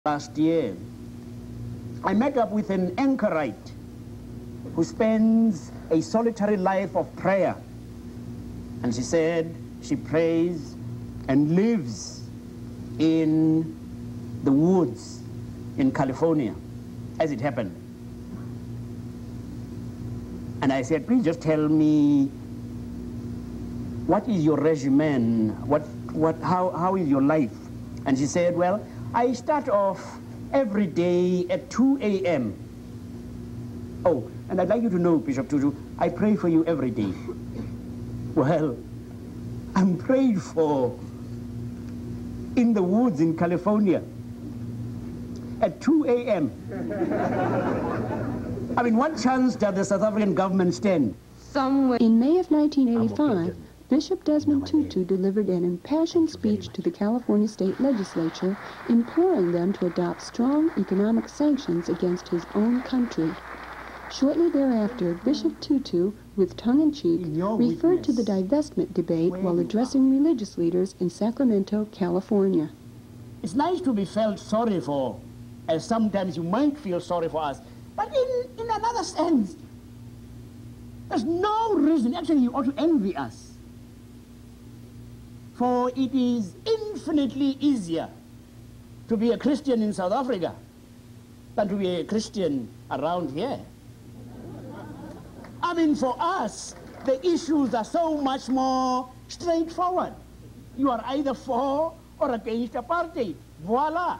1985, For Freedom In South Africa, Desmond Tutu, California Legislature, Divestment Legislation
Featuring interviews with Frank Wisner (US State Department), Herbert Beukes (South African Ambassador to the US), Randall Robinson (Free South Africa Movement), Rep William H. Gray III (Author), Senator Richard Luger, Rep. Ronald V. Dellums, Assemblywoman Maxine Waters, Leon Sullivan, Michael Watts.